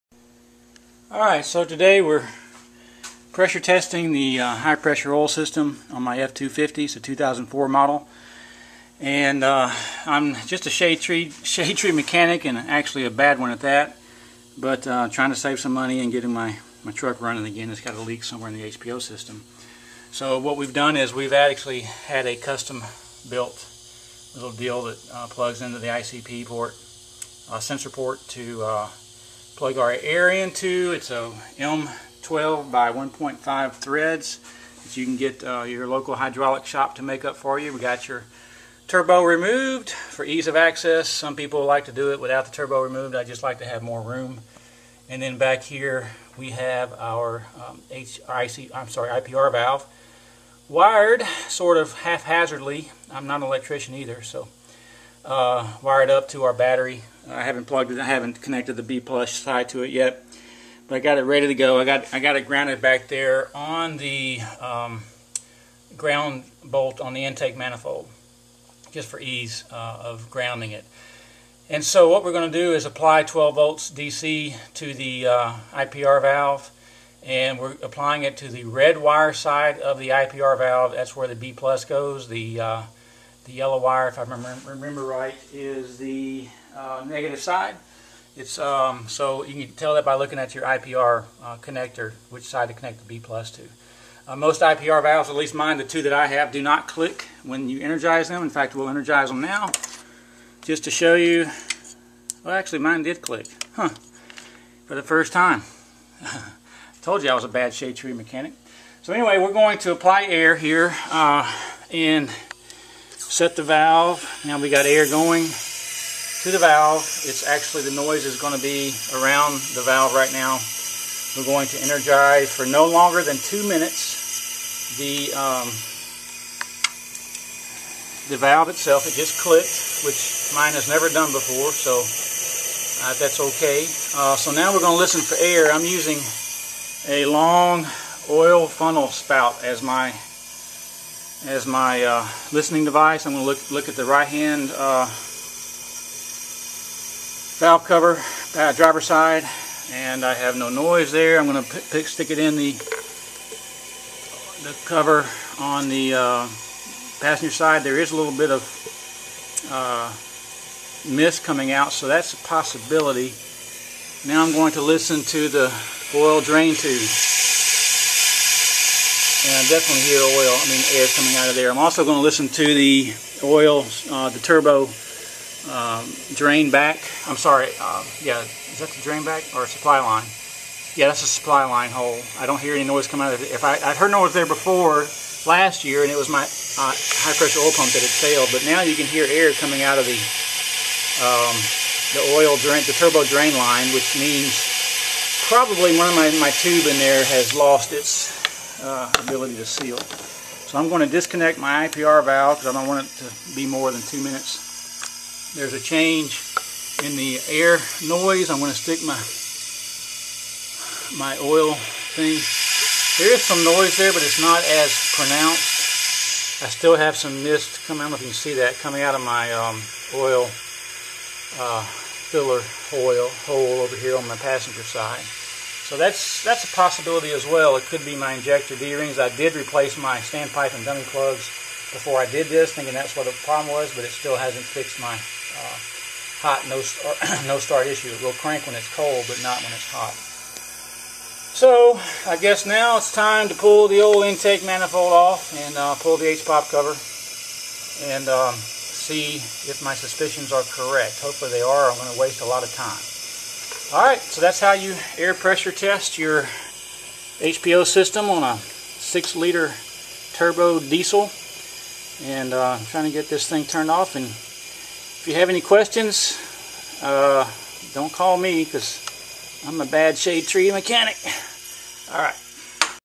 Ford 6.0 Powerstroke High Pressure Oil System Air Test
How to air test an HPO system on a 2004 F-250 6.0 Powerstroke to test for leaks.